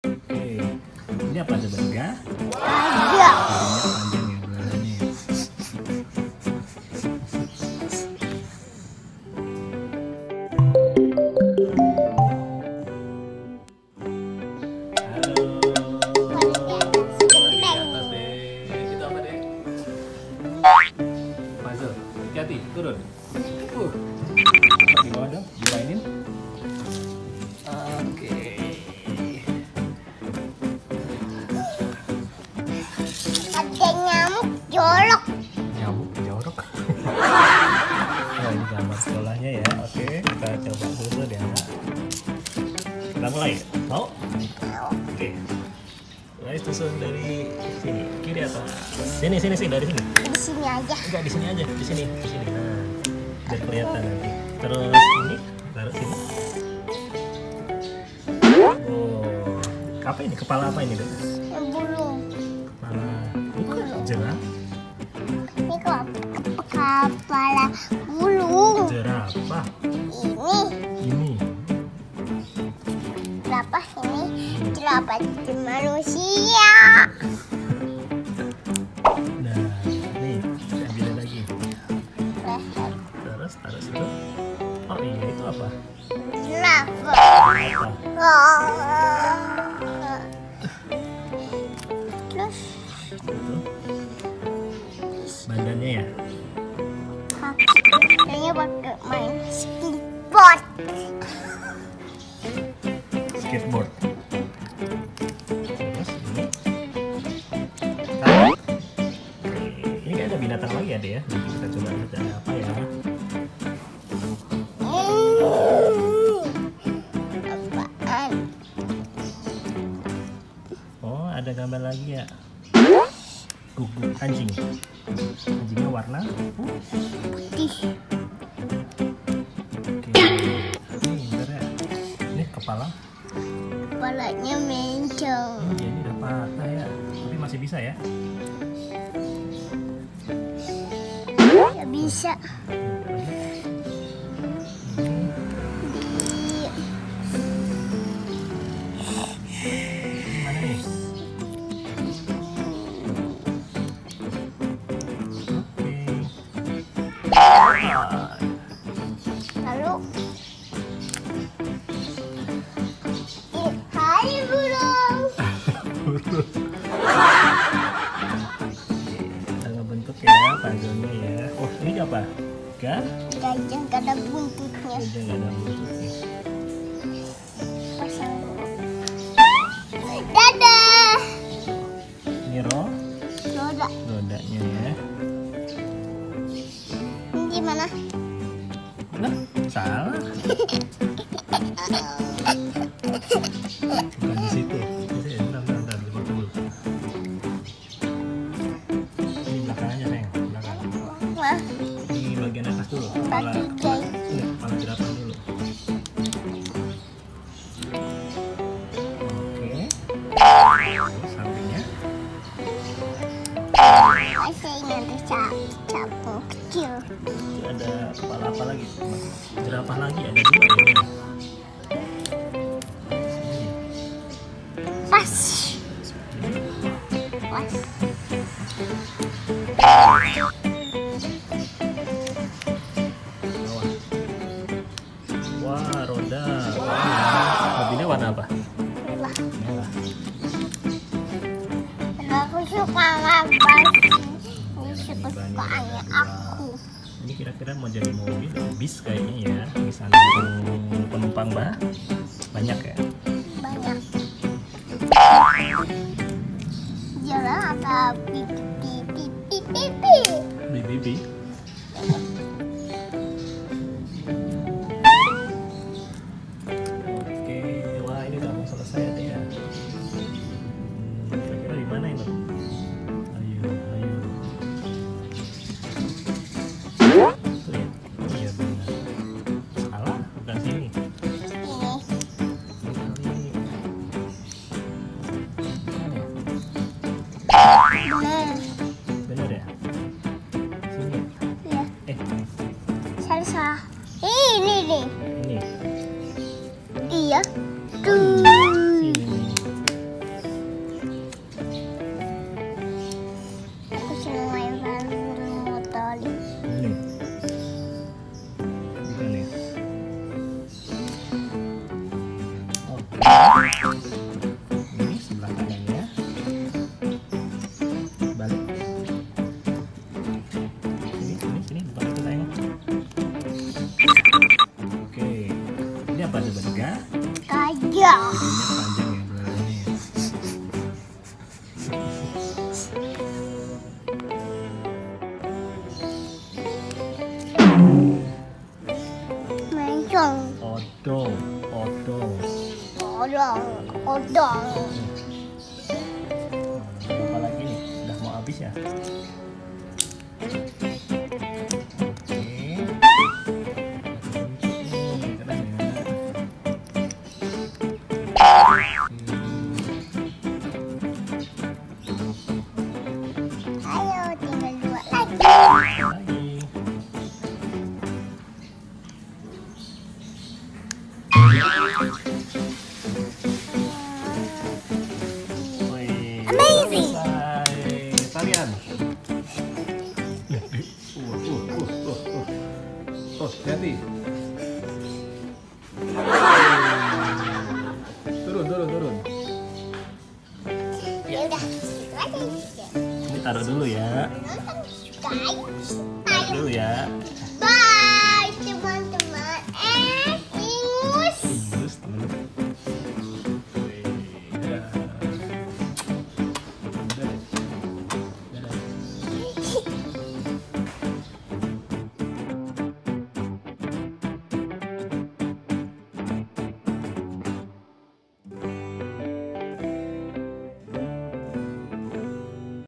Bermain Puzzle Hewan Laut | Mainan Edukasi Anak
Halo teman teman @yukkitamain di video ini kita akan bermain puzzle yang bergambar aneka hewan laut ya ada paus, hiu, ikan pari dan lain lain.
semoga kalian suka ya teman teman.